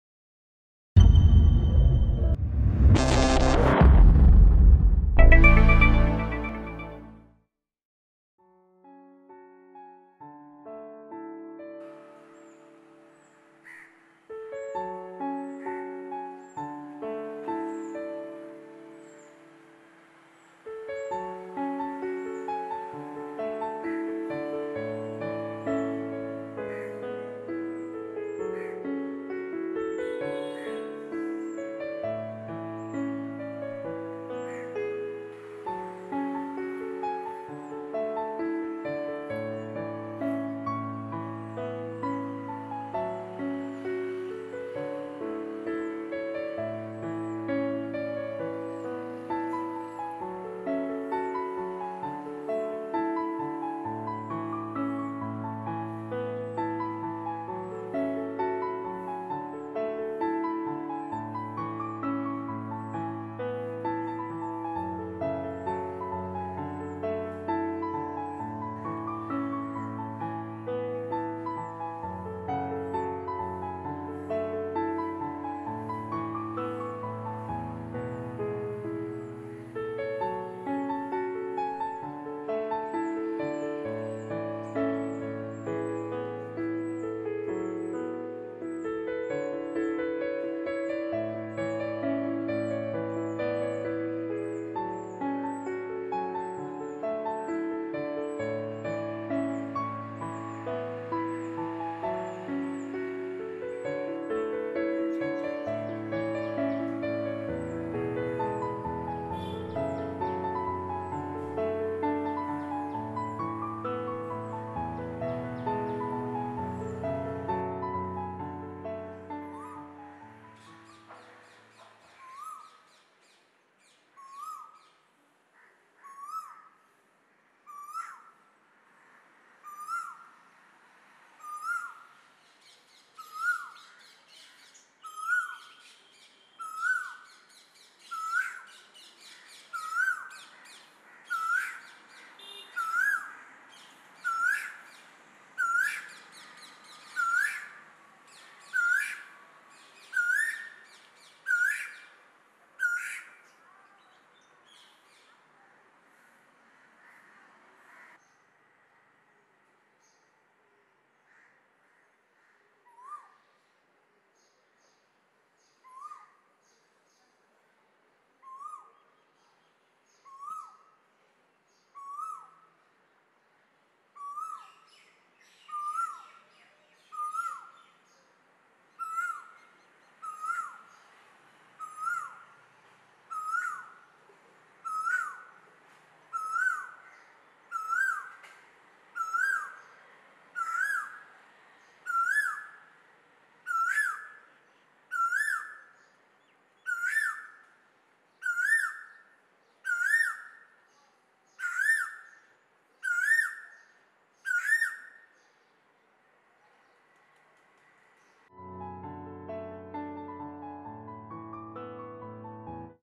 Awesome Nature-n-Wildlife of India - Asian Koel a video by Neeraj Chawla
'Awesome Nature-n-Wildlife of India' series brings an Asian koel to you on World Migratory Bird Day Today. This is to create awareness to save the bird habitat around us and help in planting more trees for the birds and saving our own environment.

' Save Nature - Save Self '. Happy Watching. !